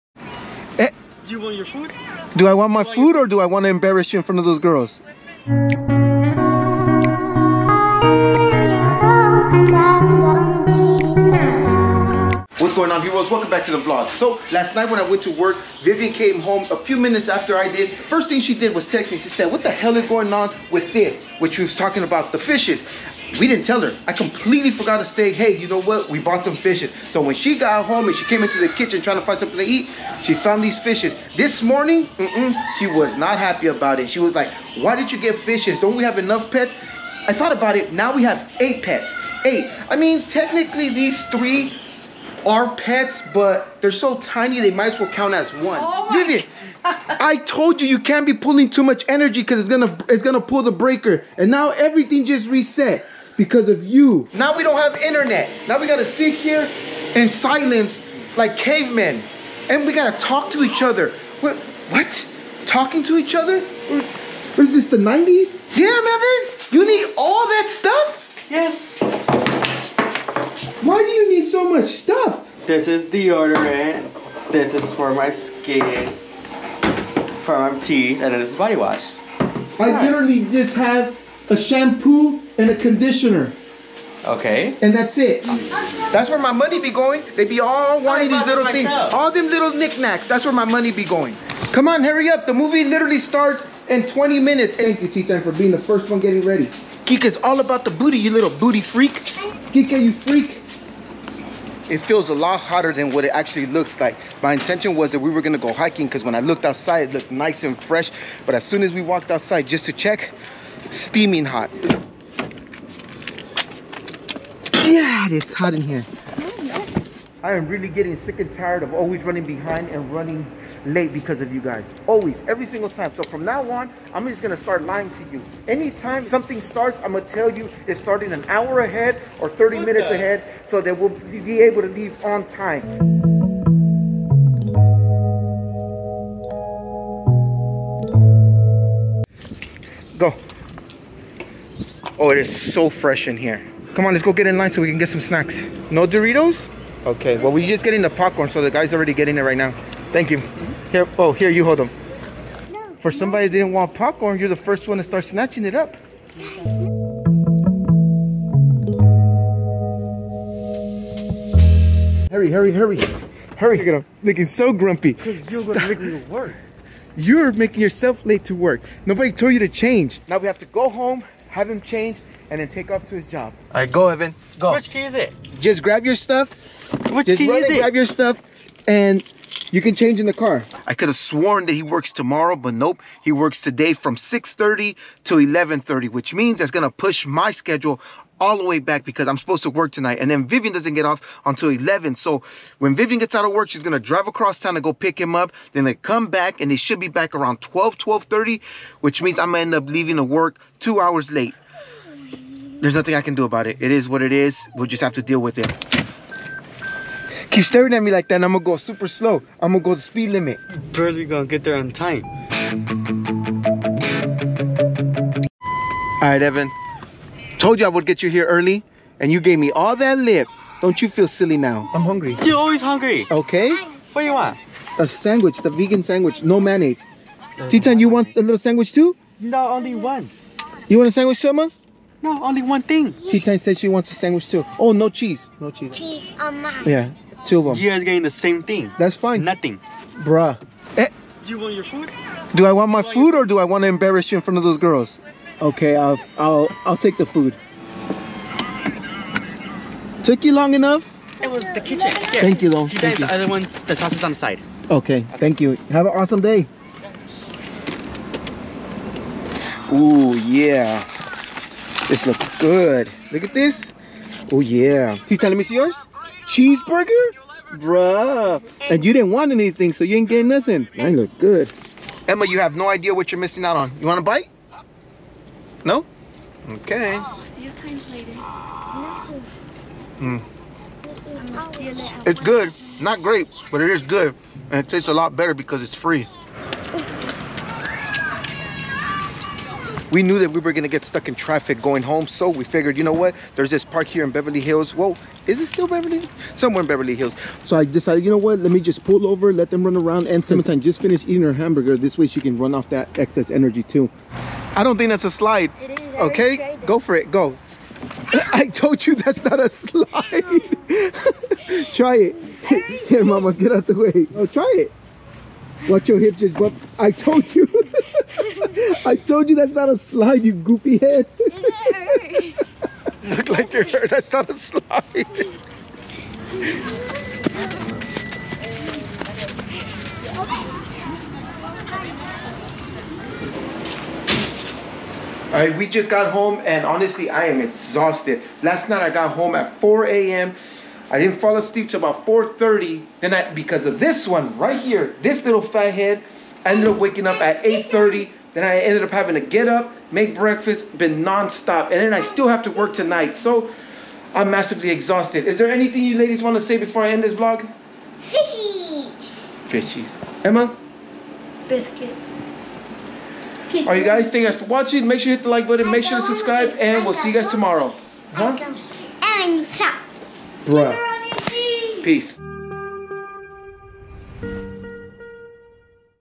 We bought 3 new pets and Vivian is not happy about it
Welcome back HEROES.
Yesterday when we bought Clementine her new pets, we forgot to mention it to Vivian so you could imagine her surprise when she got home to find that we are the owners of 8 pets now.